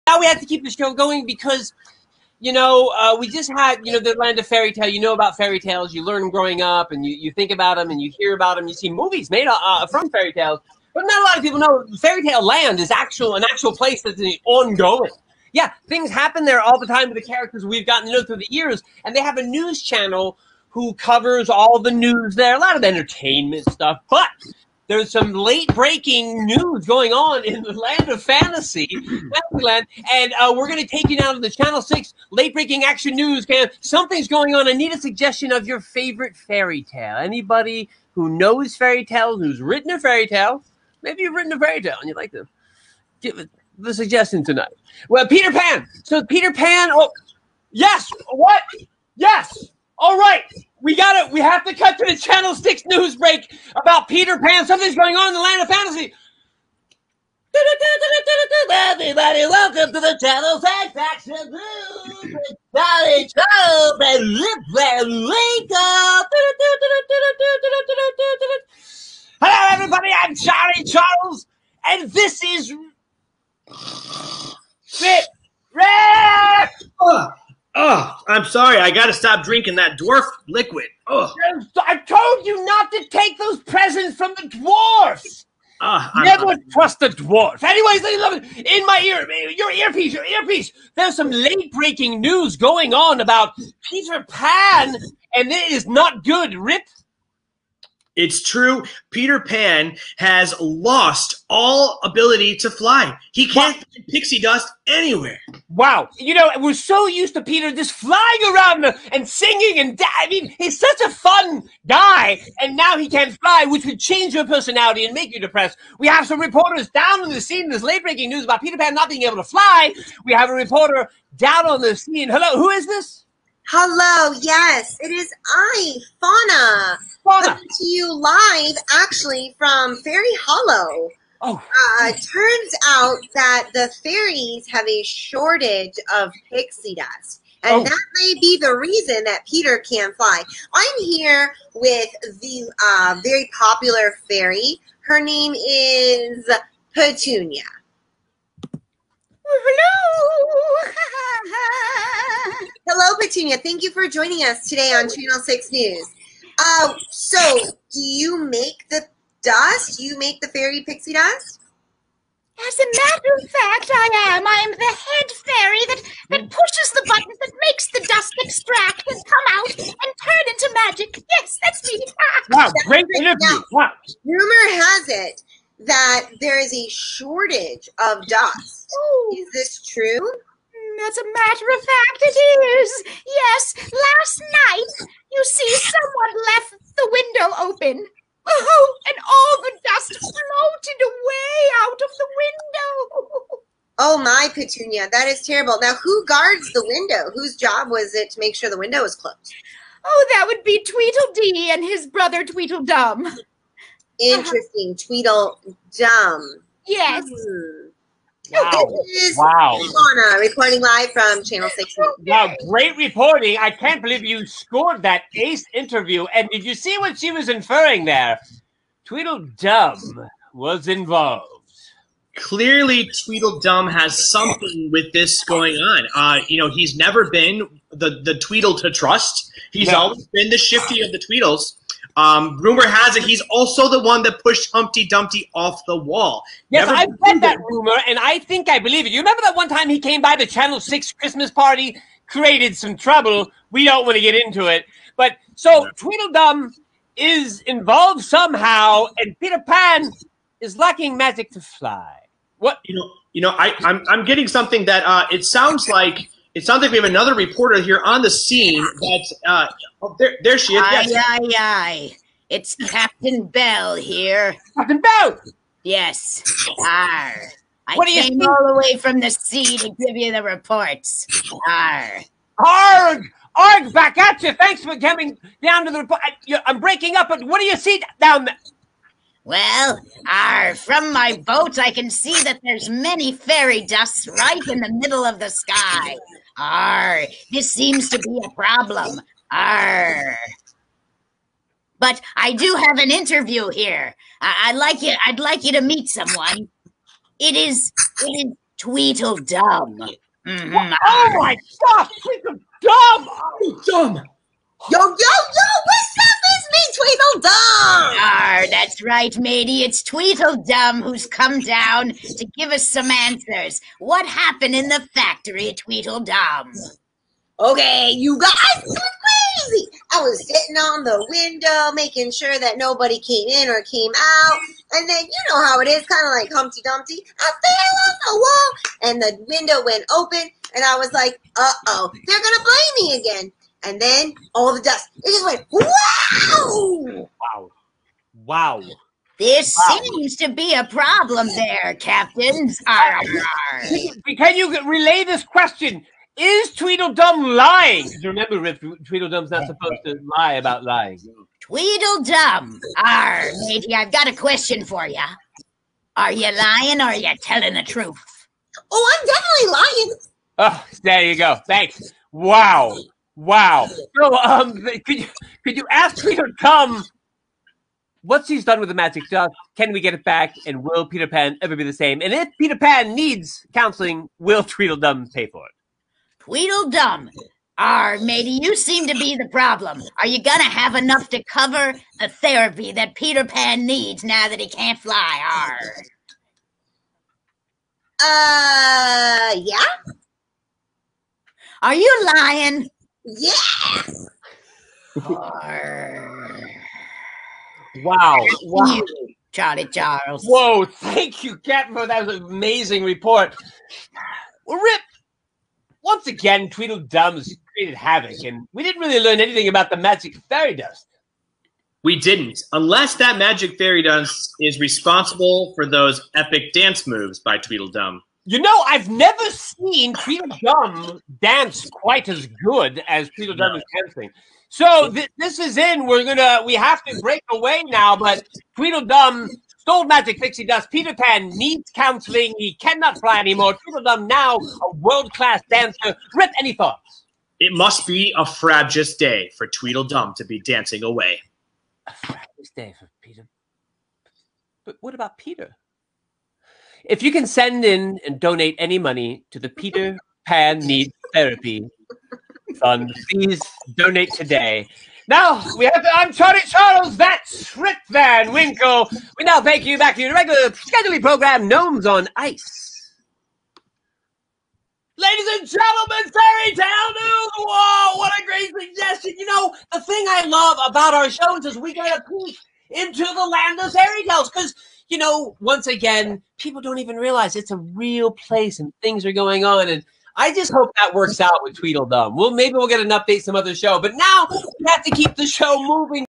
The Omelettes Comedy-Fairytale News-Peter Pan- 9/4/20
Fairytale News- Peter Pan update:
Channel 6 Late breaking news from the Land of Fairytale. ( based on suggestions from live viewers)

Omelette Improv -clip from live on-line show 9/4/20